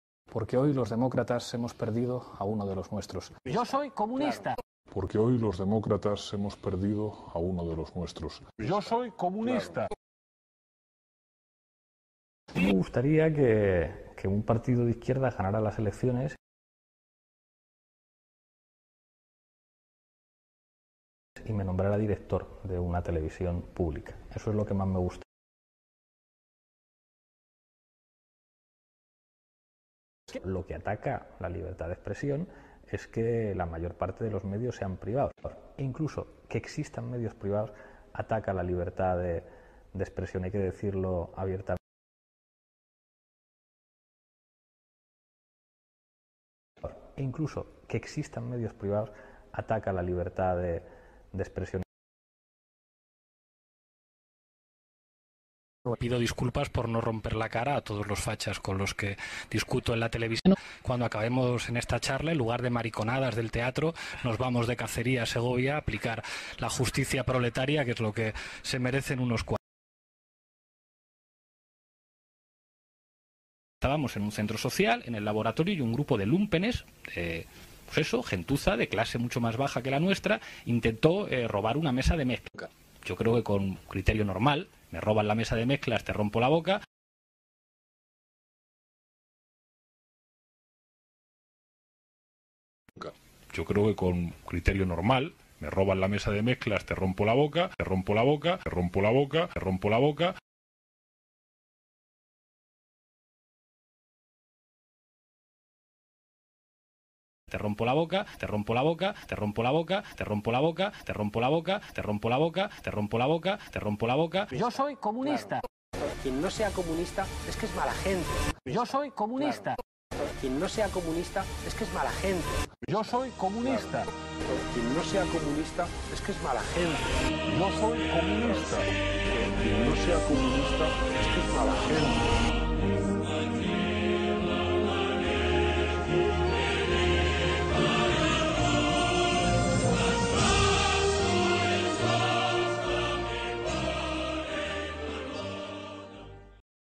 Pablo Iglesias: Potemos MIX (Majaderías selectas de PabLenin)
Selección de estupideces de Pablo Iglesias "PabLenin" (también conocido como "el marqués de Galapagar" debido a que vive en una suntuosa —e ilegal— mansión con piscina de 770.000 € ubicada en este selecto distrito madrileño) que ponen de manifiesto el carácter peligrosamente antidemocrático de este radical leninista antisistema financiado por la narcodictadura venezolana y por la dictadura teocrática islamista iraní que ahora homosexuales menores de edad en las calles